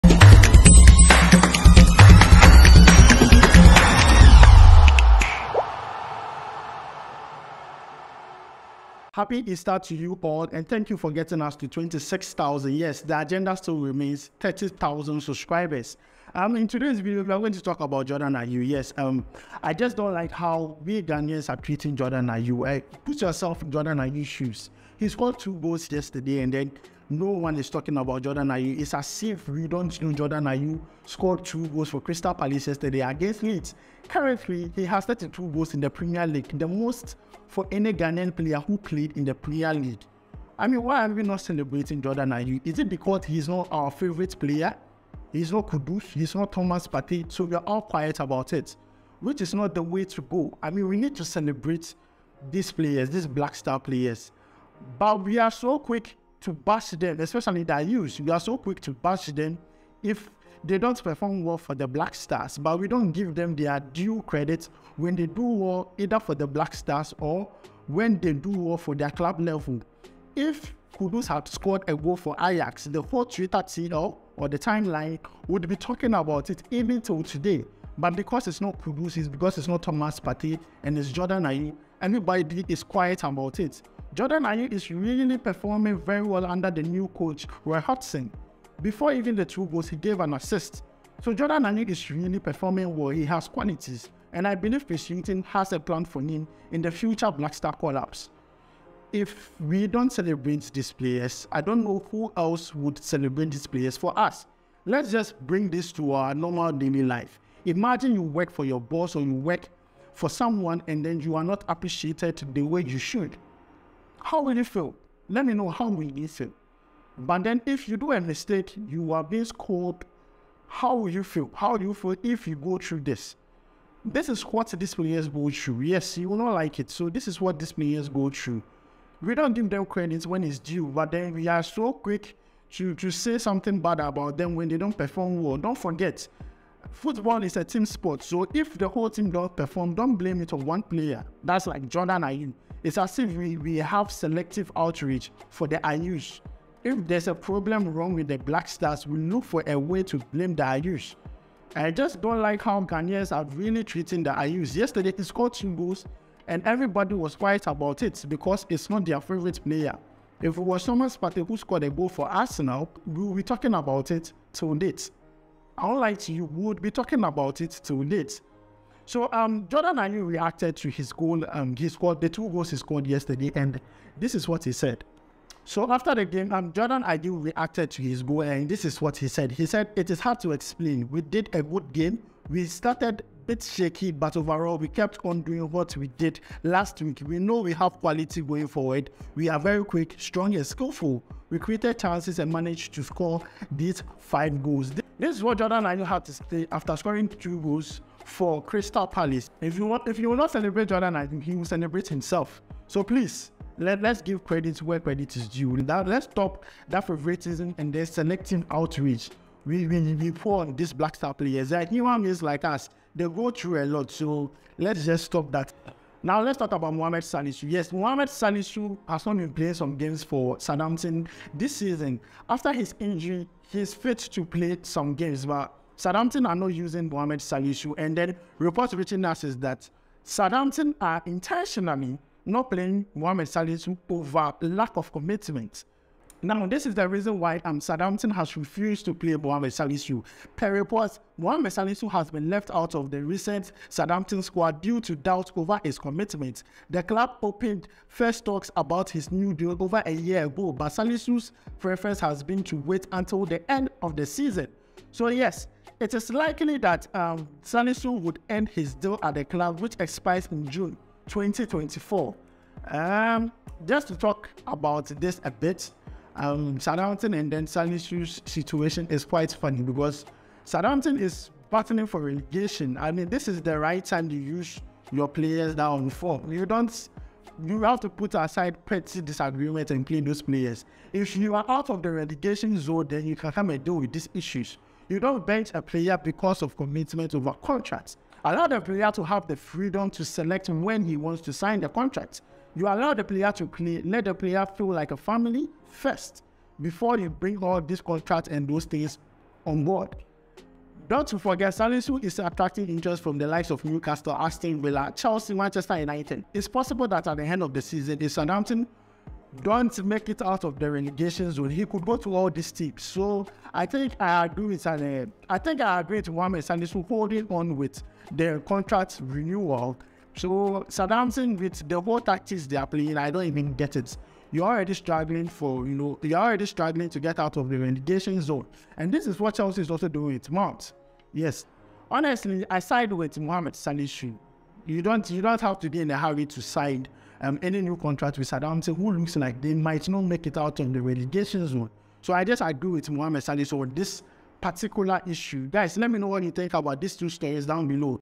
MOHAMMED SALISU IN BIG TROUBLE WITH SOUTHAMPTON & JORDAN AYEW DESRVES BETTER | BLACK STARS LATEST
Mohammed Salisu in big trouble with Southampton
And Jordan ayew deserves better from Ghanaians